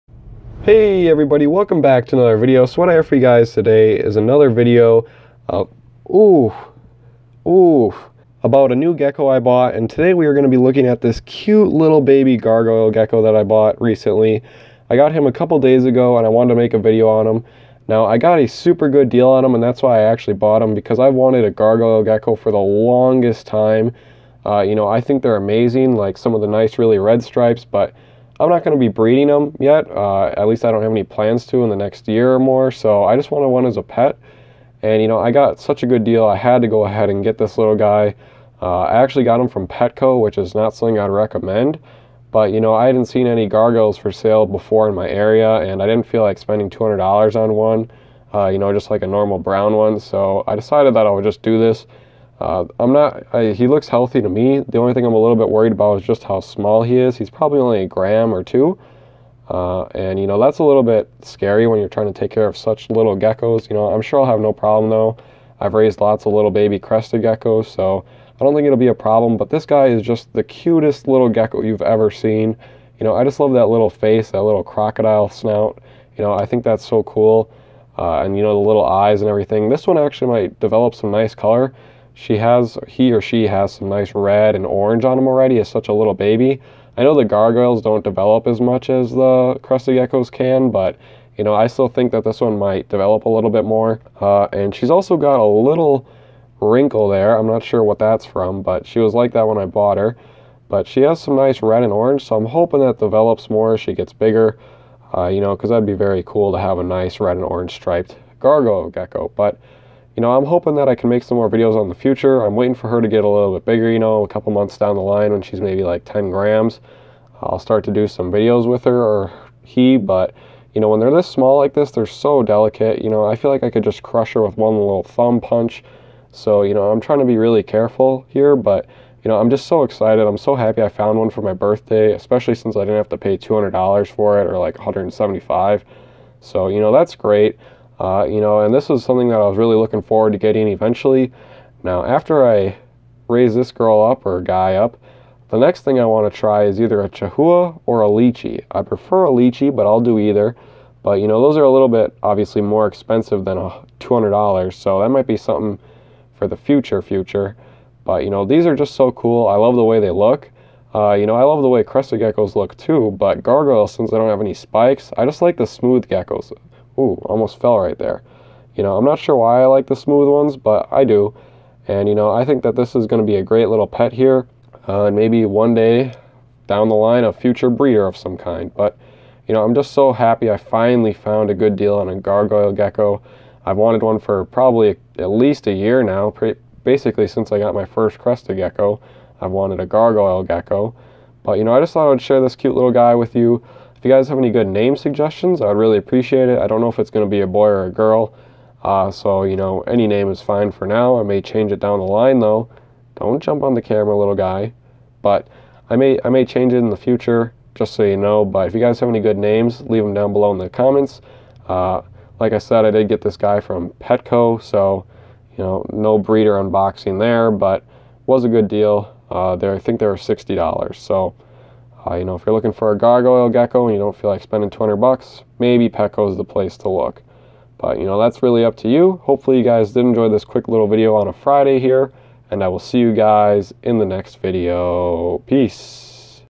I Got A New Baby Red And Orange Gargoyle Gecko!?
I can already tell this little baby is going to develop into one killer gargoyle gecko! I decided to buy this as an early birthday present!